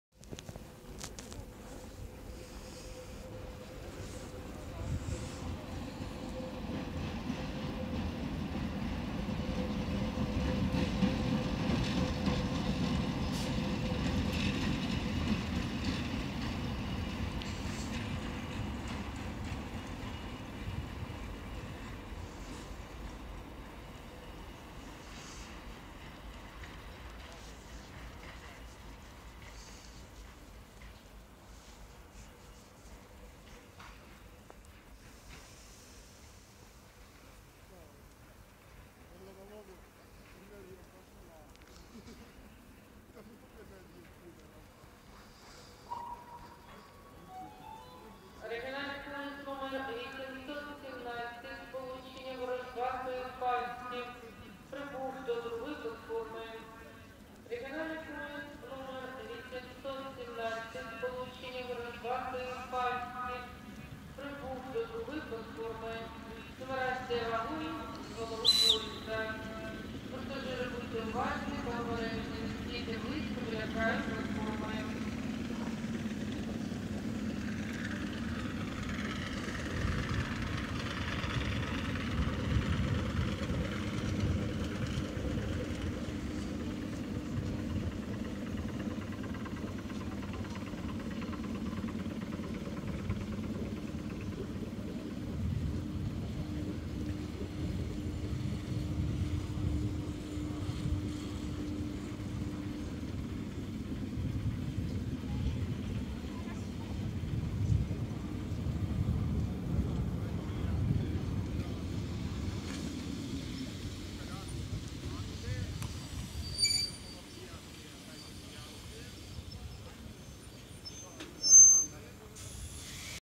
"как все добре починалось" Чме3 и Ер9м-535 817 Ворожба-Фастів прибуває в Конотоп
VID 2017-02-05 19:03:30
Дальше на перегоне Плиски-Круты из-за негабарита КС обламаем пантографы, в Нежин доедем сами медленно +1час 40 мин, далее вцепит нас вл80т-1379.

"как всё хорошо начиналось" Чмэ3 и Эр9м-535 817 Ворожба-Фастов прибывает в Конотоп